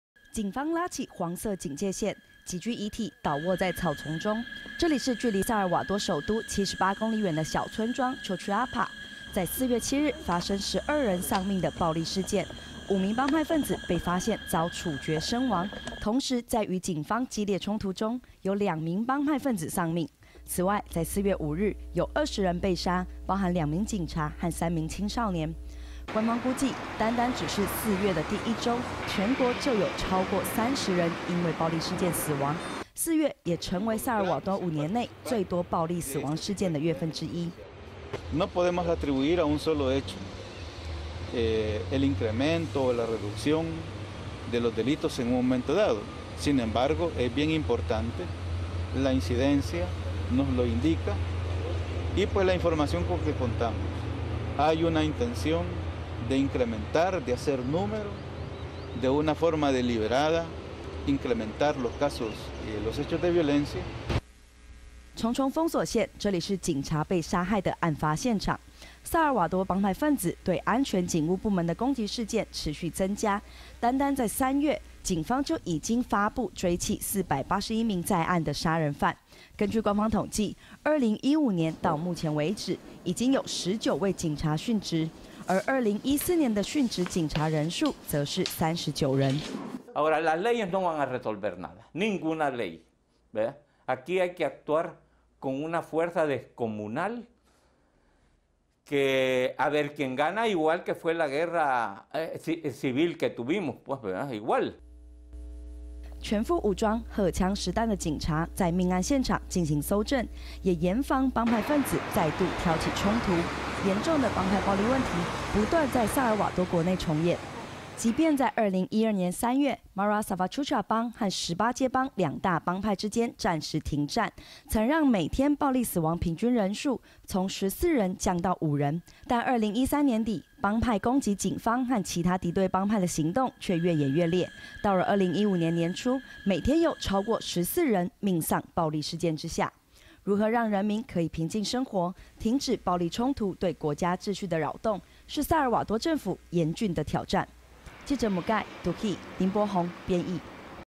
薩爾瓦多衝突不斷 居民安全亮紅燈 2015-04-10 TITV 原視新聞
警方拉起黃色警戒線，幾具遺體倒臥在草叢中。這裡是距離薩爾瓦多首都78公里遠的小村莊Chalchuapa，在4月7日發生12人喪命的暴力事件，五名幫派份子被發現遭處決身亡，同時，在與警方激烈衝突中，有兩名幫派份子喪命。此外，在4月5日，有20人被殺，包含兩名警察和三名青少年。官方估計，單單只是四月的第一週，全國就有超過30人，因為暴力事件死亡，四月也成為五年內最多暴力死亡事件的月份之一。

重重封鎖線，這裡是警察被殺害的案發現場，薩爾瓦多幫派份子對安全警務部門的攻擊事件持續增加，單單三月，警方就已經發布追緝481名在案的殺人犯。根據官方統計，2015年到目前為止，已經有19位警察殉職，而2014年的殉職警察人數則是39人。

全副武裝、荷槍實彈的警察在命案現場進行蒐證，也嚴防幫派份子再度挑起衝突，嚴重的幫派暴力問題，不斷在薩爾瓦多國內重演。

即便2012年3月，Mara Salvatrucha幫和18街幫兩大幫派之間暫時停戰，曾讓每天暴力死亡平均人數從14 降到5，但2013年底，幫派攻擊警方和其他敵對幫派的行動卻越演越烈，到了2015年年初，每天有超過14人，命喪暴力事件下。如何讓人民可以平靜生活，停止暴力衝突對國家秩序的擾動，是薩爾瓦多政府嚴峻的挑戰。